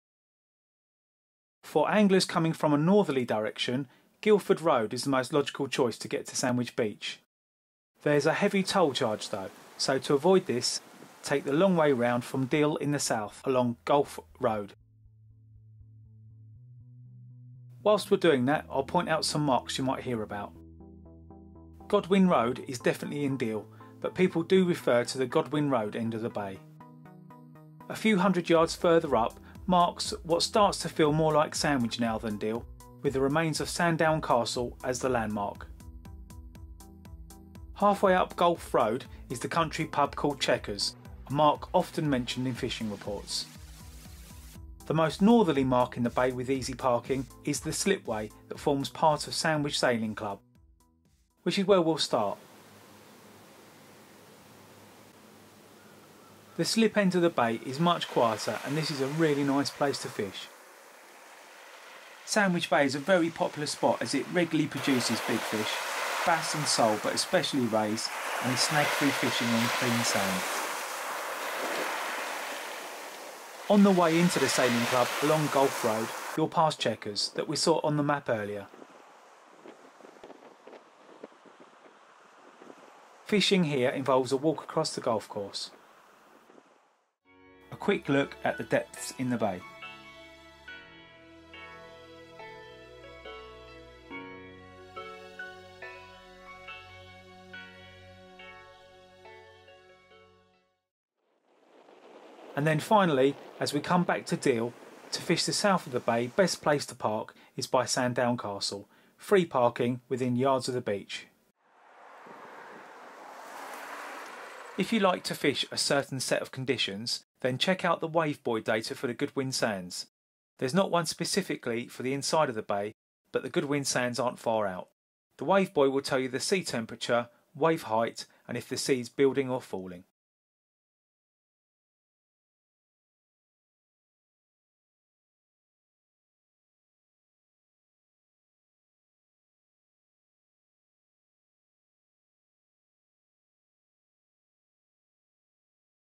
Sandwich Bay - beach fishing marks, South East Coast, England, Britain
Beach fishing marks in Sandwich Bay, Kent.

for more angling video guides to
English South East Coast shore fishing marks.

Sandwich Bay has a few spots often seen in fishing reports - the slipway, Chequers and Sandown Castle.

This video covers all of them, including where to park, so the shore angler can go to Sandwich having a couple of back up options.